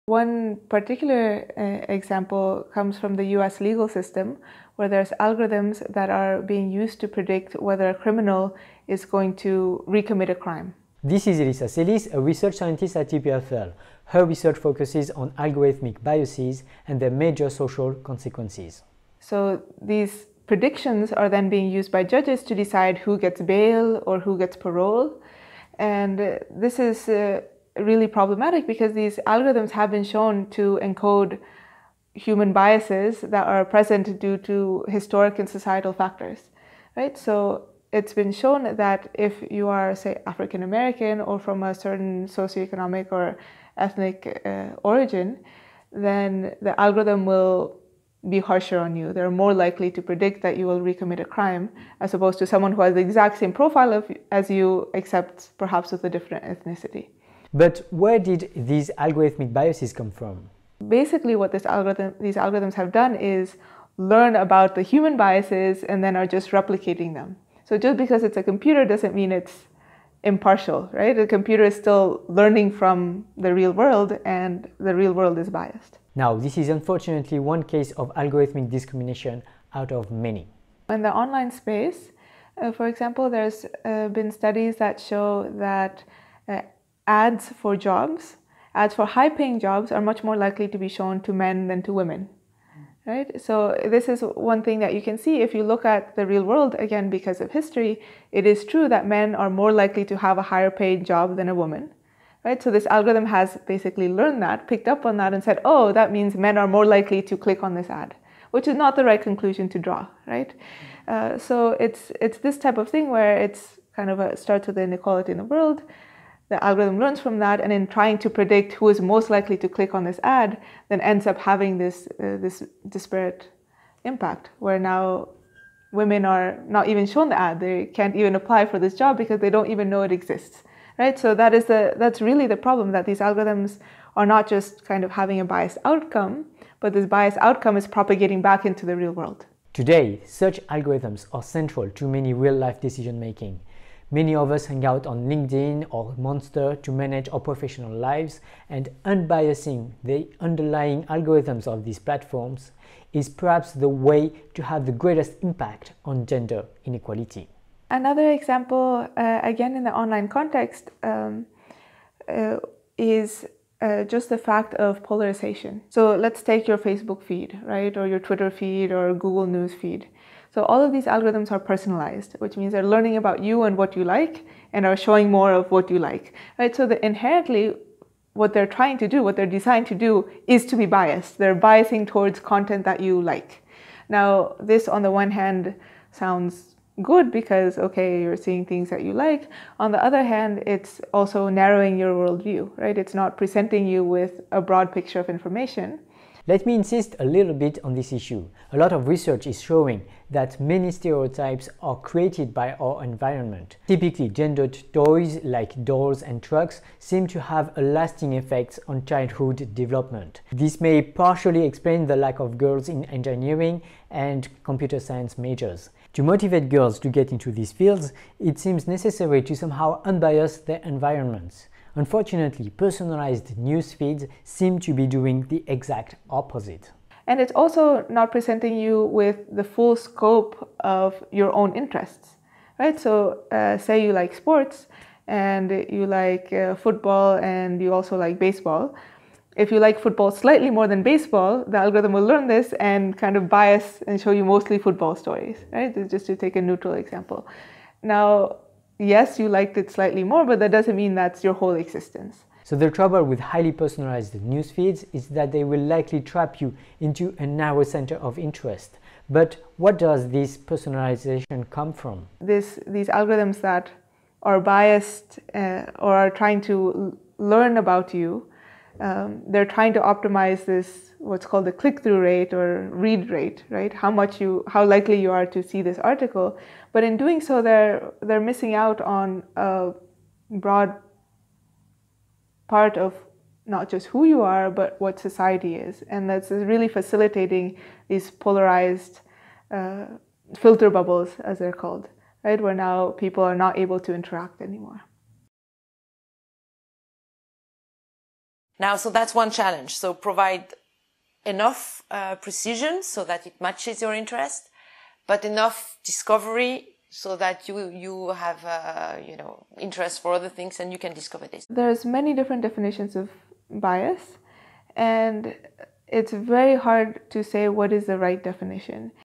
3 Cases of Discriminative AIs (ft. Elisa Celis)
This video discusses 3 cases of discriminative AIs out of many. It features Dr. Elisa Celis, research scientist at EPFL.